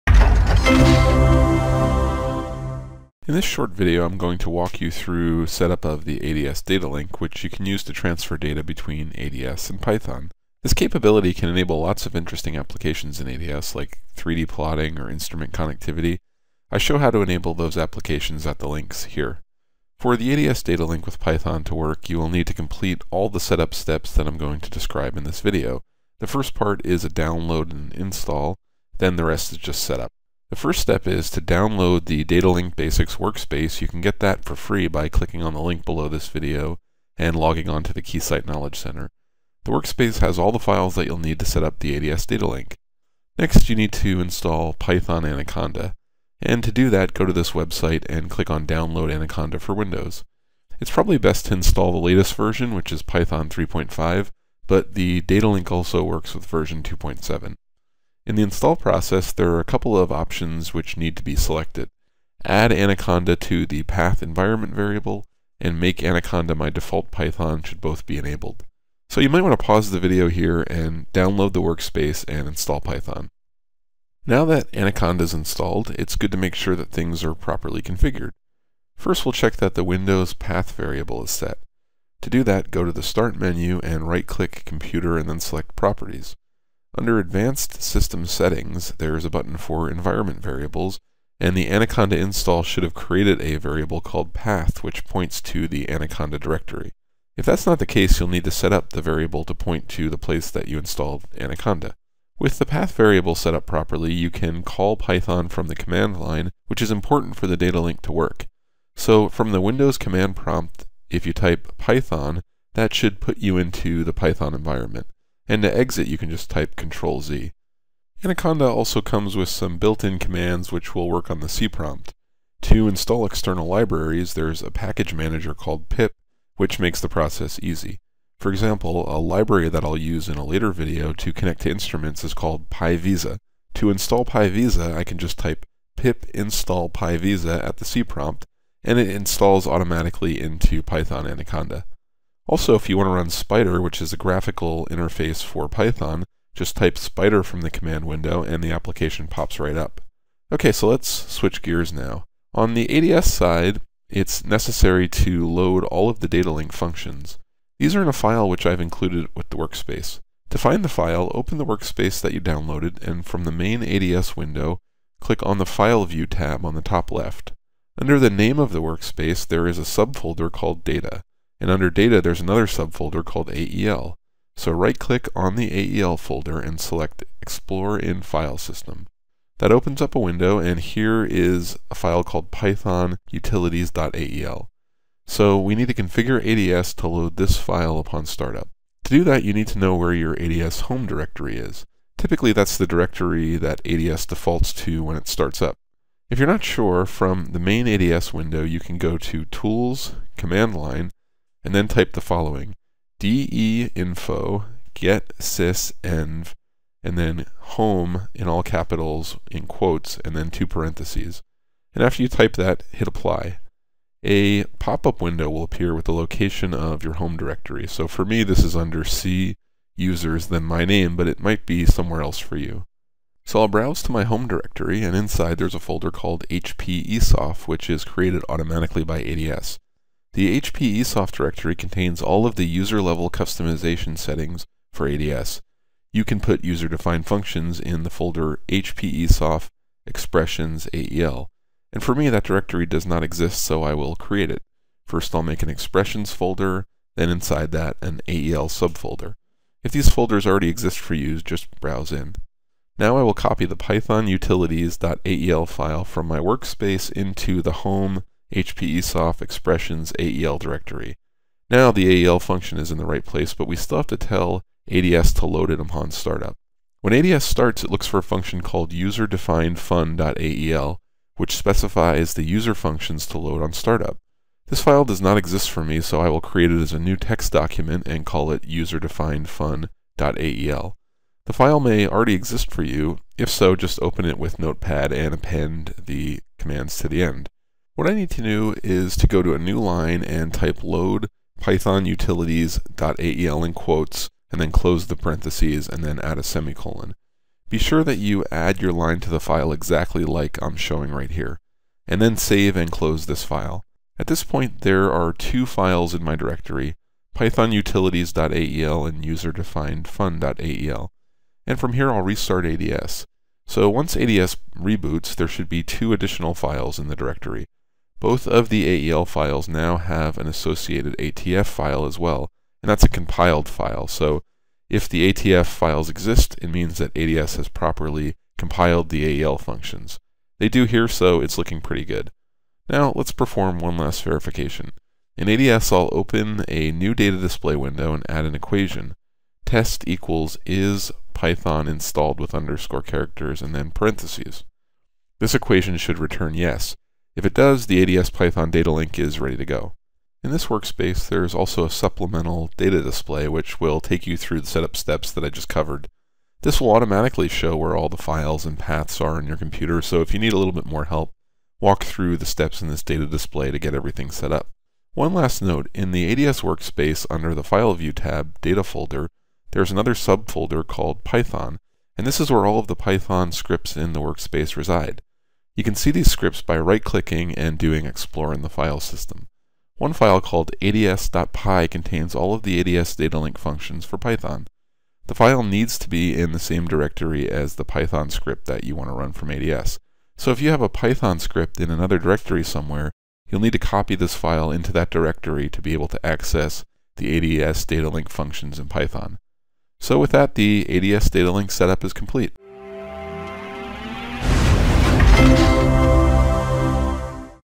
ADS Data Link Setup
The Datalink feature has been updated since this video, and is now fully integrated into ADS for the 2019 release!

This video describes the steps to set up a data link between Keysight Advanced Design System (ADS) and Python Anaconda
(registered users only)